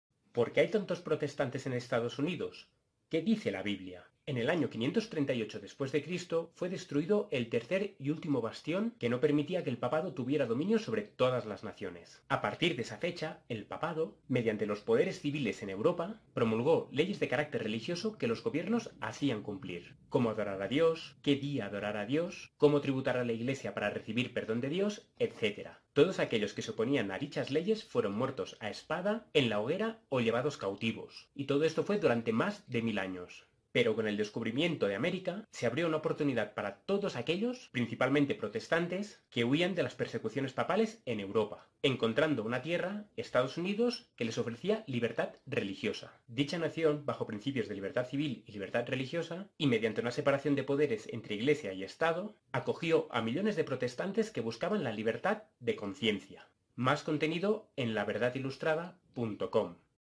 🎯🙏 EN 1 MINUTO ¿Por qué hay tantos protestantes en EEUU? ¿Qué dice la Biblia?
Mensajes cristianos de 1 minuto: Estados Unidos es considerado un país con fuertes raíces protestantes, ¿pero a qué se debe? ¿Cómo un país tan lejos de la cuna del protestantismo se convirtió en un bastión en defensa de la libertad de los mismos?
Con el descubrimiento de América se abrió una oportunidad para todos aquellos, principalmente protestantes, que huían de las persecuciones papales en Europa (Apocalipsis 12:13-15), encontrando una tierra, EE.UU (Apocalipsis 12:16), que ofrecía libertad religiosa.

Si nuestro contenido te ha sido de bendición, ayúdanos para continuar nuestra labor.

España: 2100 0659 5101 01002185 (La Caixa)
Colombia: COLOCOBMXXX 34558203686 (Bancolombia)

¡Muchas gracias!